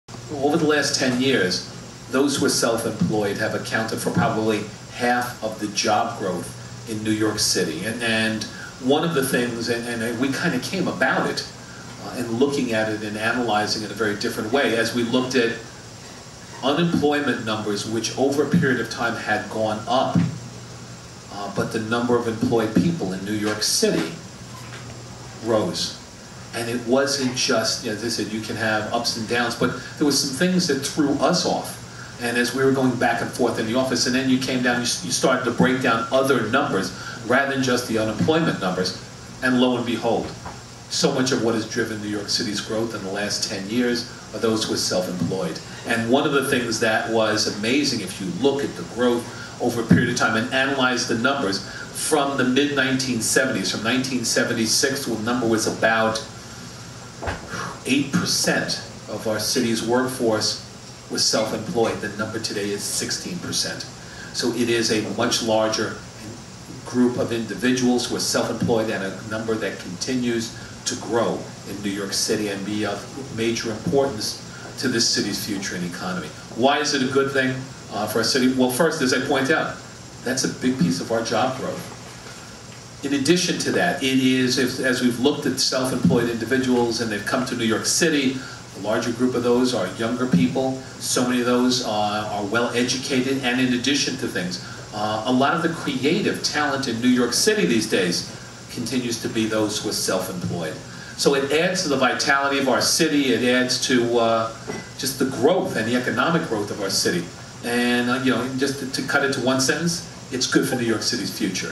Comptroller Thompson on freelancers in NYC's economy
Freelancers are driving job and economic growth in New York City.